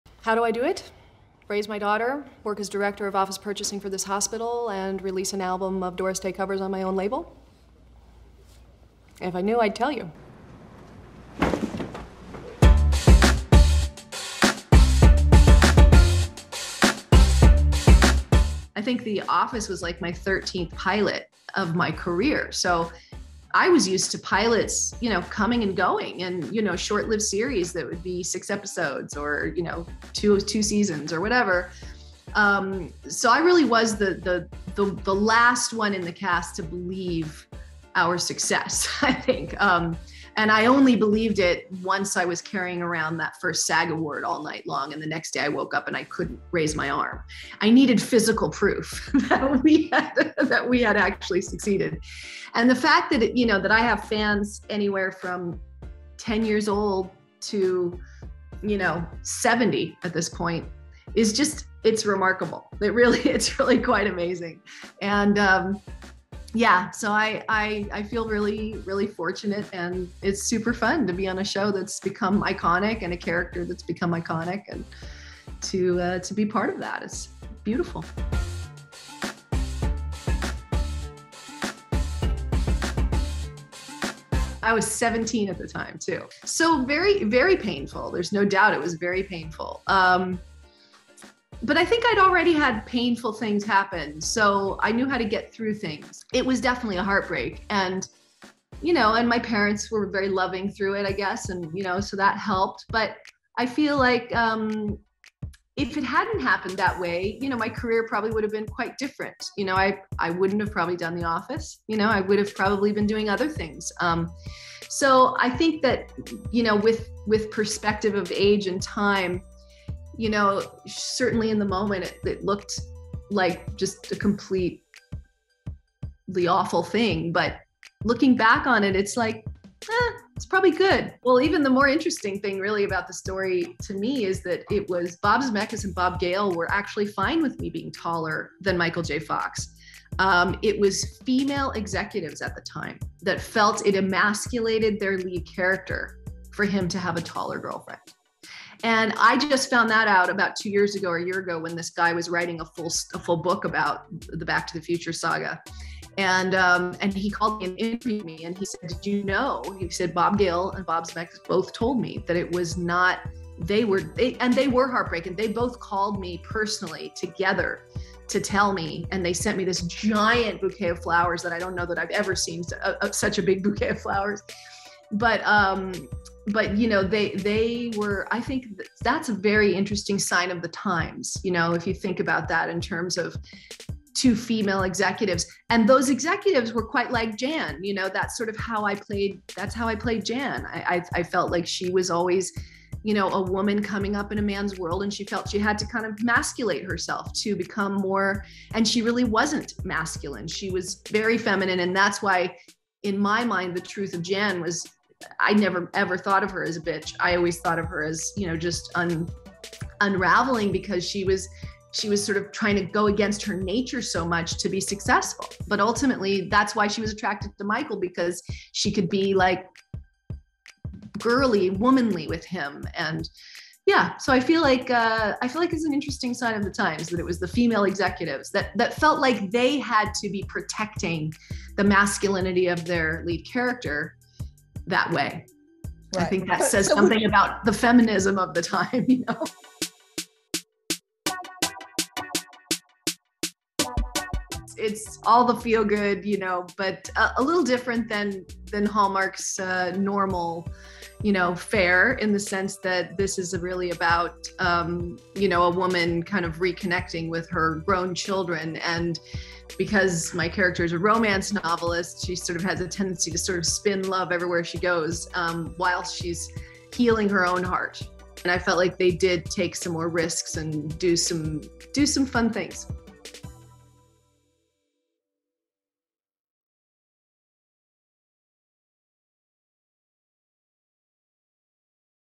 Melora Hardin on losing ‘Back to the Future’ role at 17 | Page Six Exclusive Interview
Melora Hardin admits that it was devastating being axed from “Back to the Future” for simply being taller than the film’s star, Michael J. Fox.

Hardin was 17 when she was originally cast as Jennifer Parker in the classic ’80s film alongside Eric Stoltz, who was set to play Marty McFly. But when Stoltz was replaced with Fox, Hardin also got the boot.